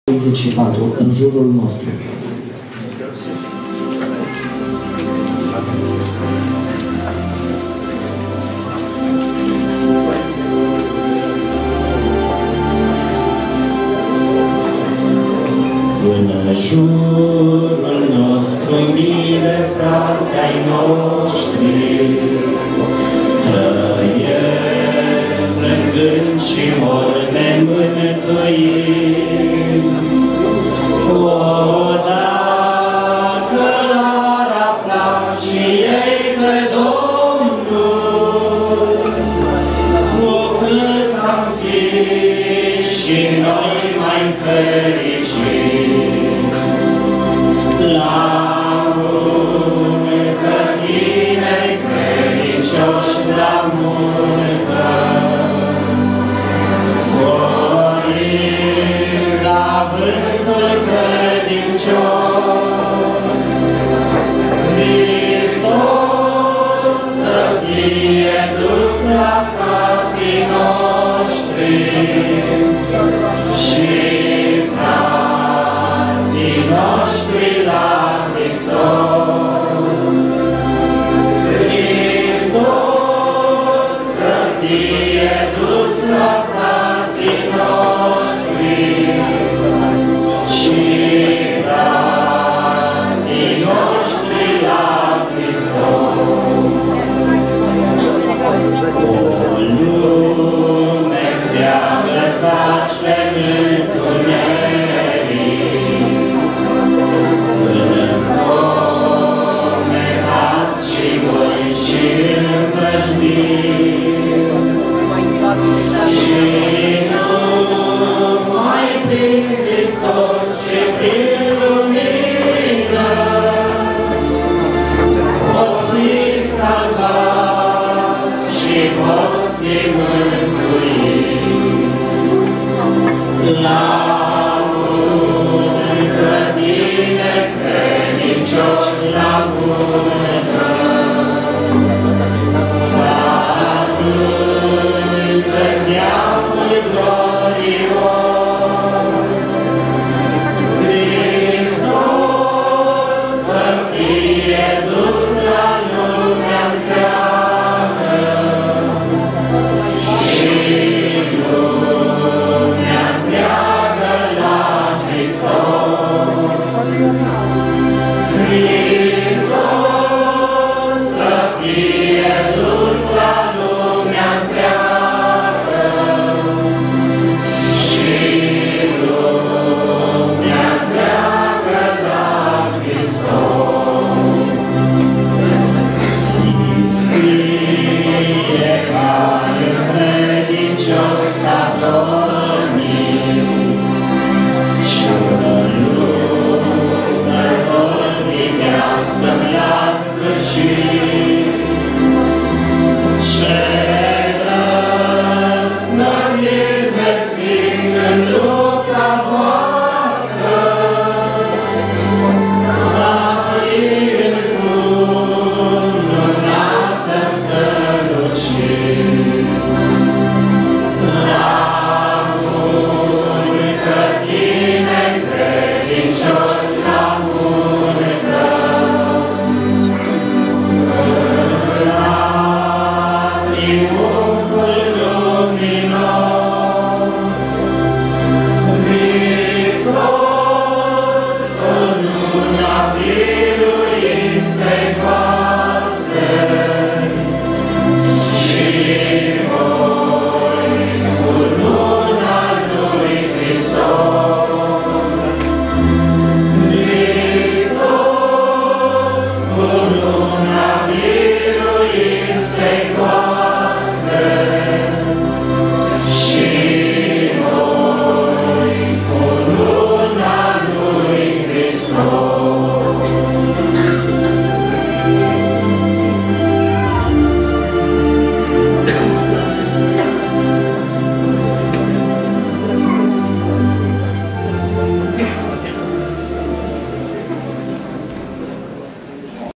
În jurul nostru (Sibiu, 7 iun. 2014)
De la întâlnirea tinerilor din Oastea Domnului cu ocazia Praznicului "Pogorârea Duhului Sfânt" (Sibiu, 7 iun. 2014)